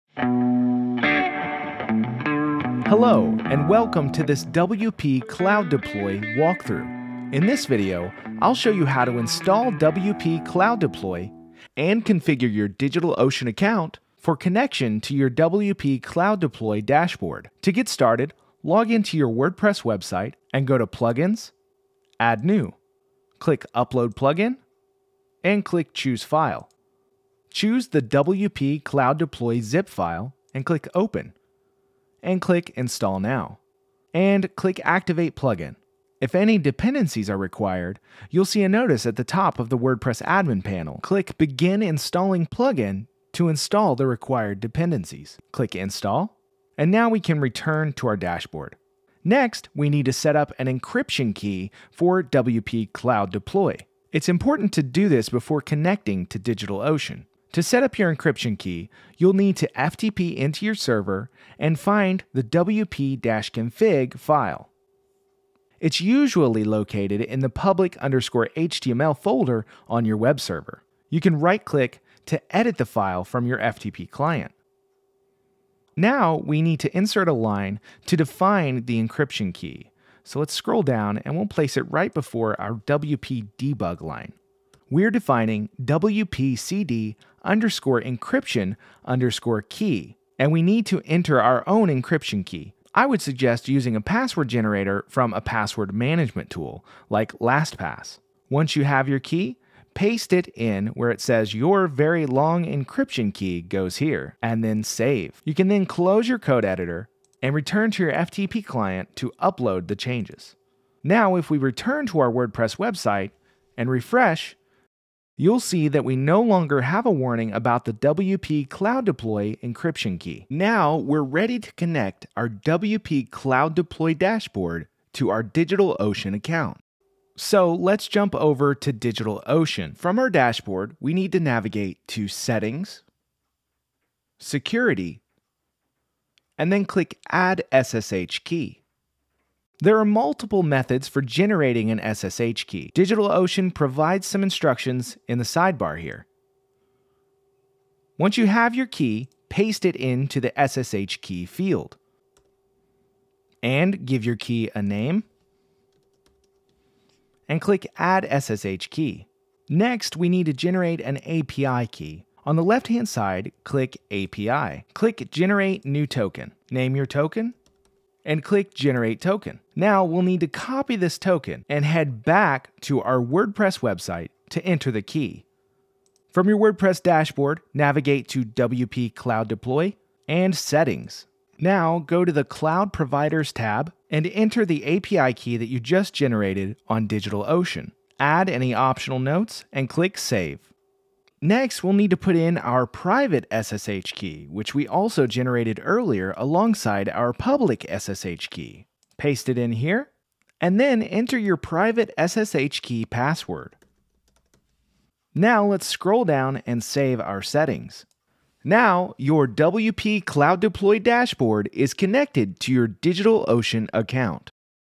How To Install WPCloudDeploy
A quick walk-through of how to install WPCloudDeploy - from uploading the plugin to your WordPress site to obtaining and installing API keys from DigitalOcean.